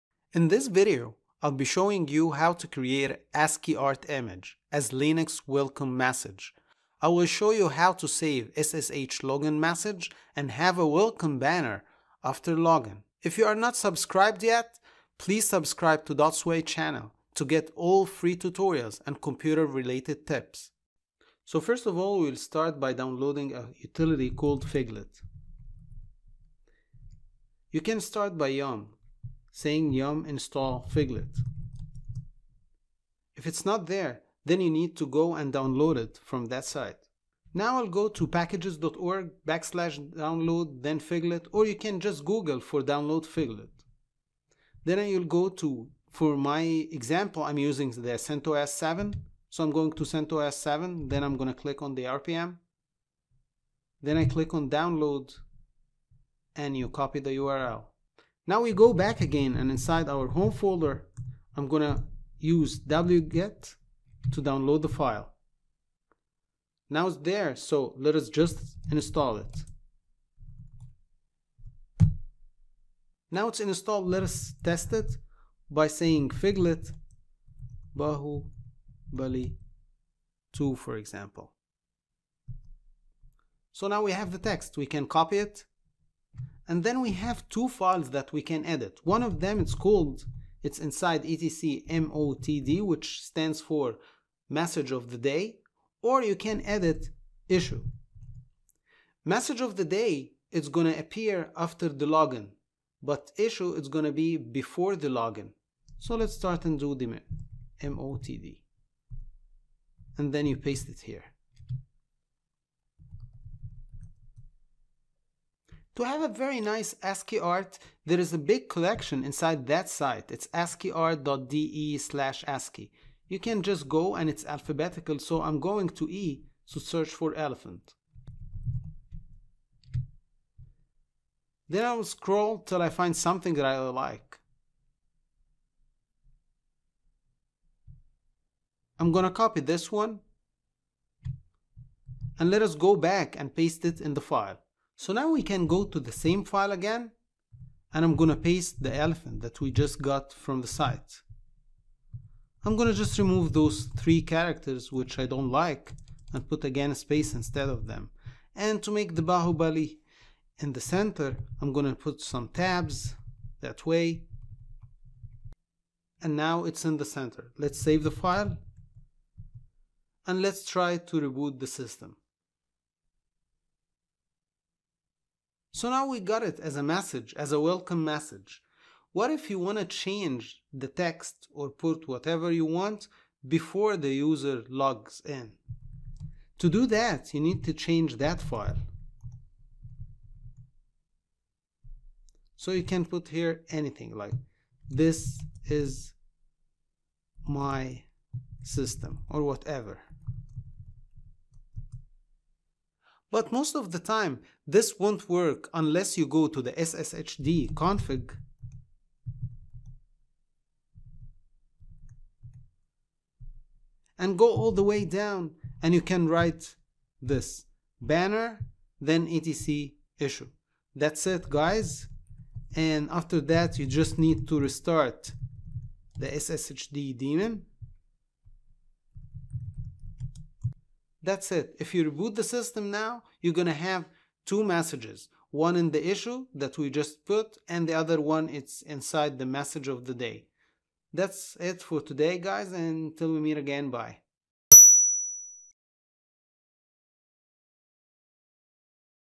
Linux Tutorial : Bahubali Ascii Art Image SSH Banner as Linux Welcome Message
In this tutorial i will show you how to use Ascii art image as a linux welcome message in both /etc/motd and /etc/issue

First we need to have Bahubali text as an Ascii art which will be better than normal text, for this i will be using a utility called Figlet which is a graphic text generator, Figlet is a text to ascii art conversion can make your welcome screen much nicer by having several feature and different output types. It's one funny linux command!
Download Figlet the graphic text generator
Automatically

Now install Figlet using:

rpm -i package_name.rpm

Now you can test Figlet big font generator
Figlet bahubali 2

Now you can copy the ascii art image and use it as Linux login message 'ssh login message'

vi /etc/motd
paste the ascii art here
What is motd ?

This is the message of the day, the content of this file will appear after successful ssh login.
Also you can use the generated ascii text as a ssh welcome message by editing the etc issue file.

Activate the ssh banner

vi /etc/ssh/sshd_config
add

Banner /etc/issue
Save /etc/issue file

On RHEL 7
systemctl restart sshd
On RHEL 5/6
service sshd restart

You can use sites like ascii-art to find also ascii art image for your Linux login message.